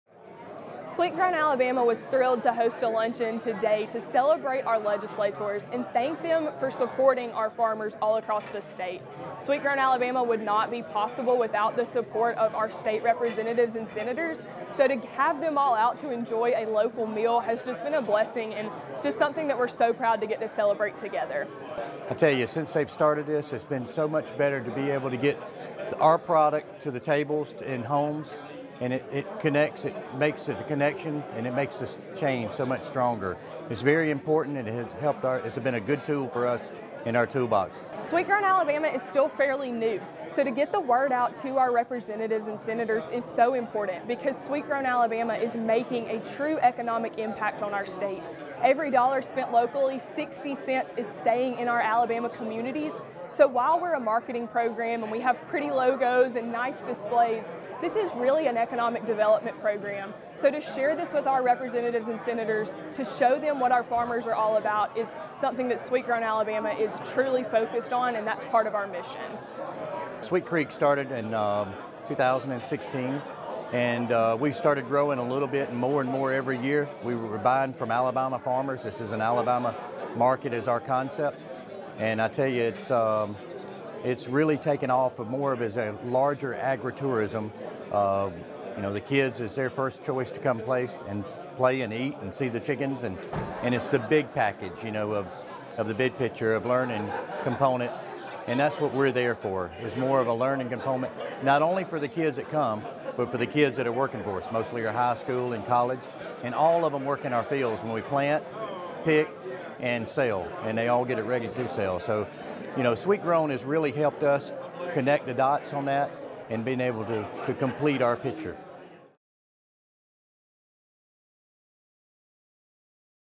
Sweet Grown Alabama Legislative Luncheon
Sweet Grown Alabama Director Ellie Watson and Alabama Rep. Reed Ingram, R-Pike Road, talk about the state agricultural branding program during the 2022 Sweet Grown Alabama Legislative Luncheon, featuring Alabama-grown foods. Ingram is a Sweet Grown Alabama supporter and owner of SweetCreek Farm Market.